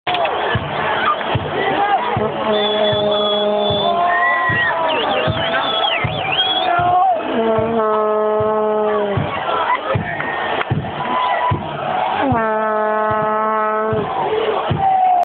Chivas GD game with the porra
at the coliseum in los angeles for a Chivas Guadalajara game